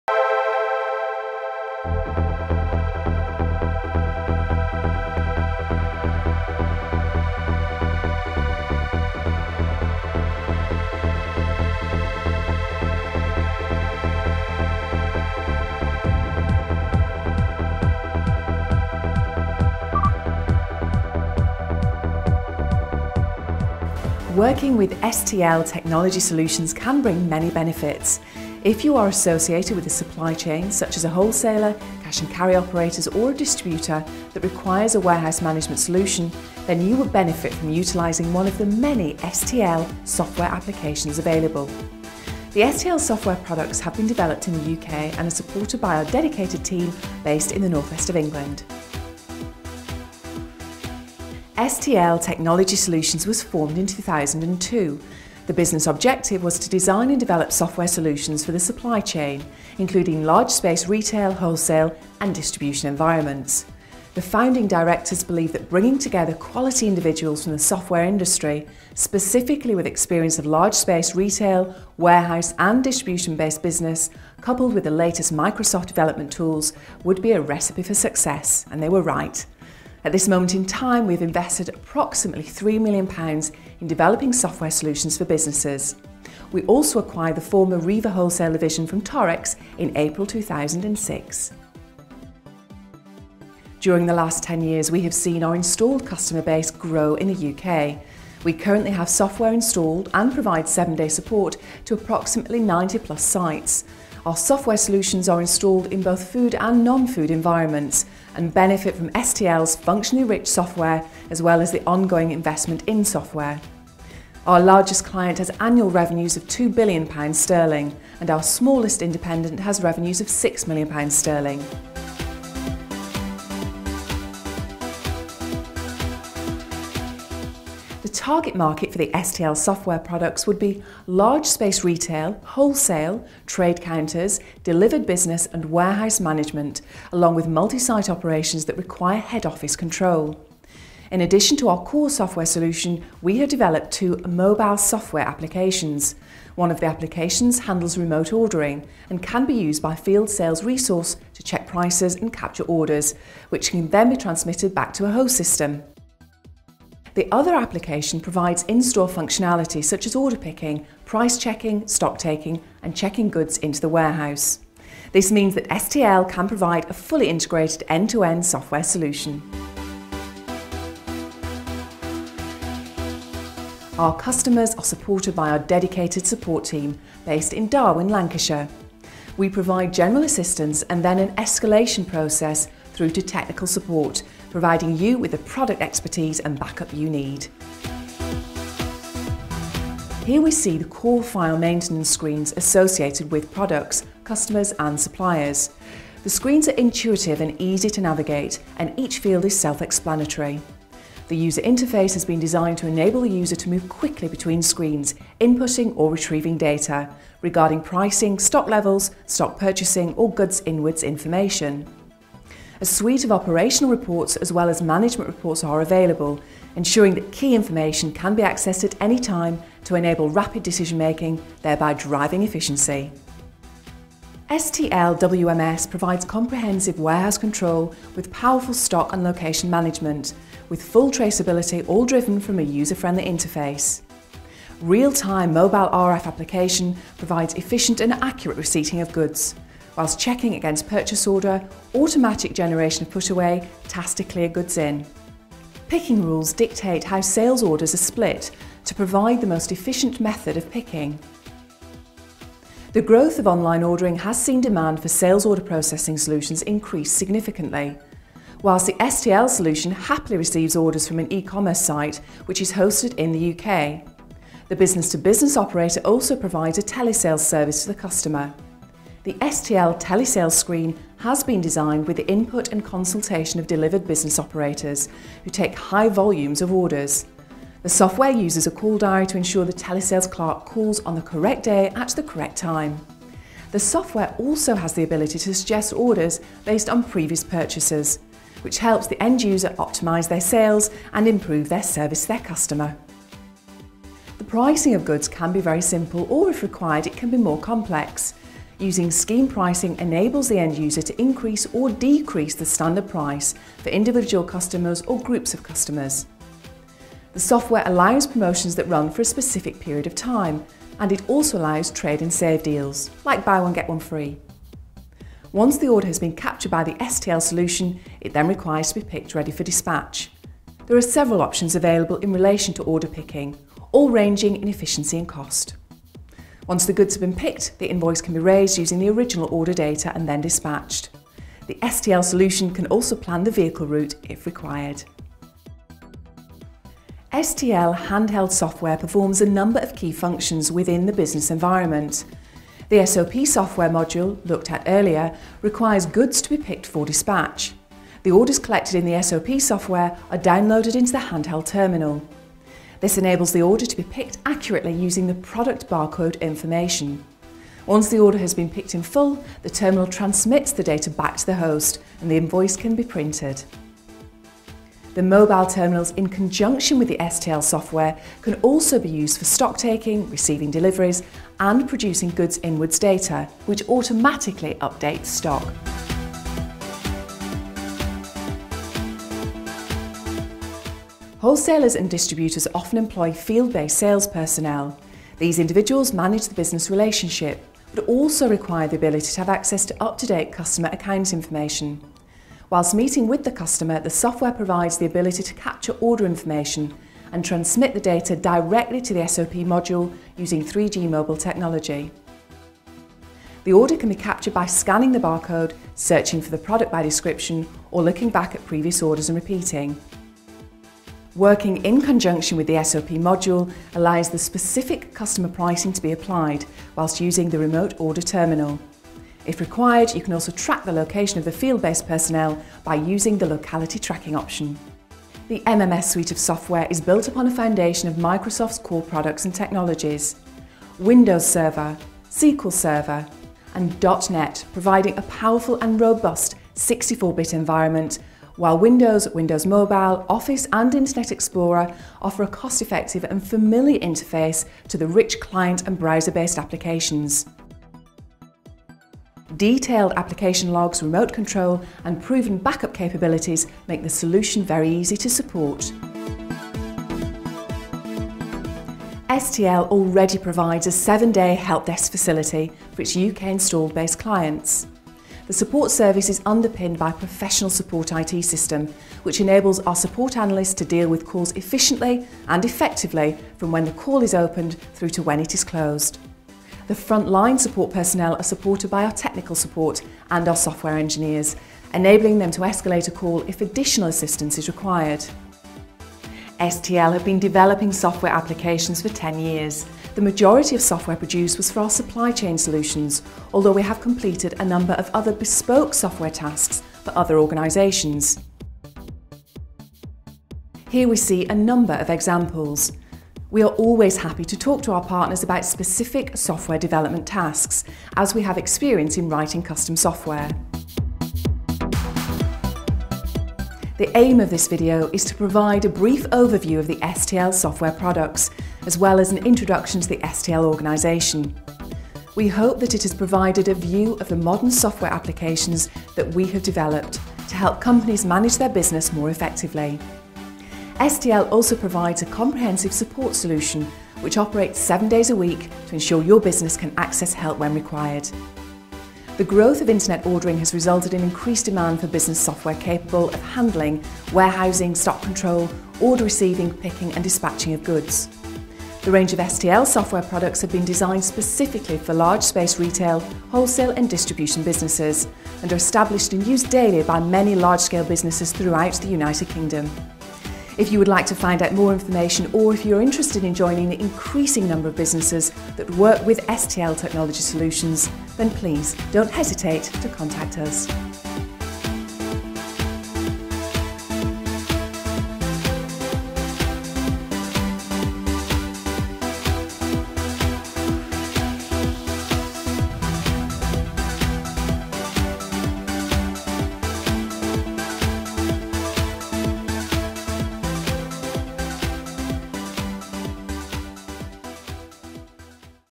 STL company & product overview.m4v
This video provides an overview of STL Technology Solutions Ltd and its software products which are installed in cash & carry and distribution businesses.